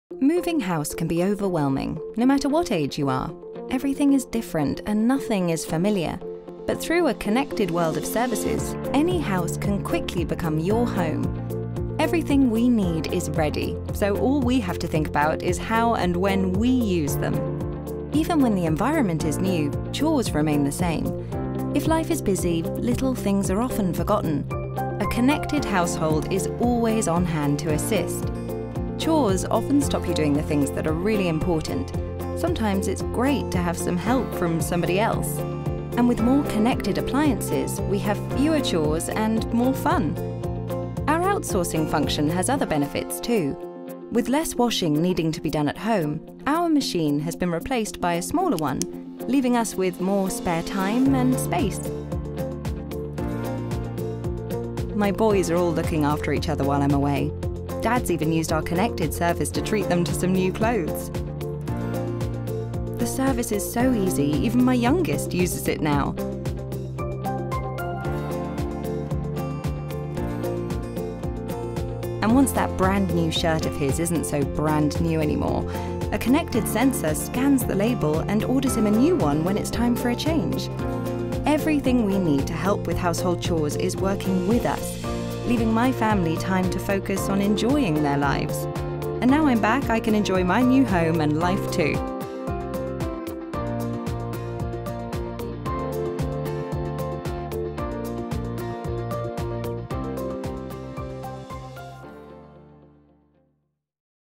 Visions for future cities and homes “Connecting services for household chores” - Hitachi
New services reduce the burden of household chores.